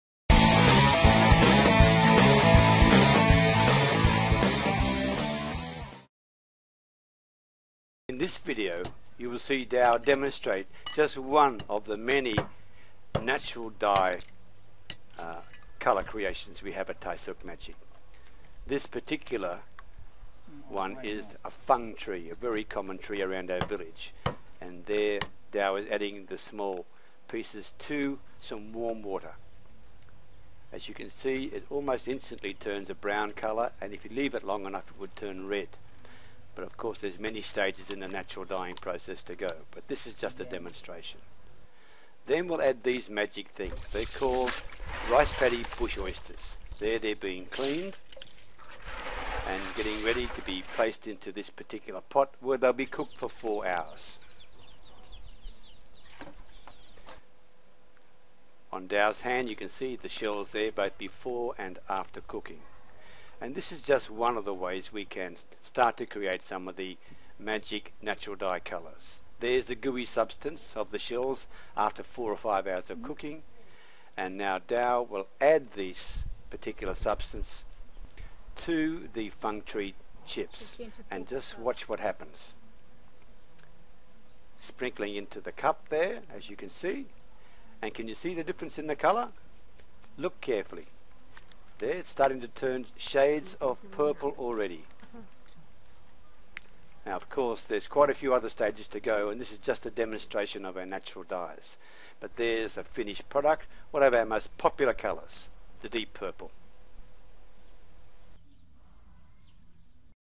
Amazing Natural Dyes Part 1 of 5 - Creating Natural Dyes
This is part 1 of a series we have created to show you more about one of our passions - natural dyes. All our Thai silk creations are totally chemical free because we ONLY use 100% natural dyes.

Our completely natural silks are the result of using some of these magical natural ingredients.

In this video you will see Dao demonstrate just one of the many natural dye colour creations we have at Thai Silk Magic.

This particular one is about the Fung tree.  A very common tree around our village and a superb natural dye ingredient for application to natural silk.

You will see Dao adding the small pieces of the Fung tree to some warm water amd watch what happens with this 100% natural dye ingredient.

0:28  As you can see it almost instantly turns a brown colour and if you leave it long enough it would turn red.

But of course there are many stages in the natural dyeing process to go. This is just a demonstration.

1:20 Check out what happens when we add the magical additional natural ingredients this natural dye material.

They're called rice padi bush oysters. There they are being cleaned and getting ready to be placed into a pot where they'll be cooked for 4 hours.

Here you will see on Dao's hand the shells there both before and after cooking.

This is just one of many ways we can start to create some of the magic natural dye colours.

Did you see what happend to to the colour here? .Look carefully.

There it's starting to turn shades of purple already.

Now of course they are quite a few other stages to go and this is just a demonstration
of our natural dyes.

But we show one of our finished natural dyes created for this video.

It's of our most popular natural dye colours. The deep purple natural dye.

Look out for the other 4 parts of our series on natural dyes and natural silks coming in the next few weeks. Please leave us your comments below - we will always respond.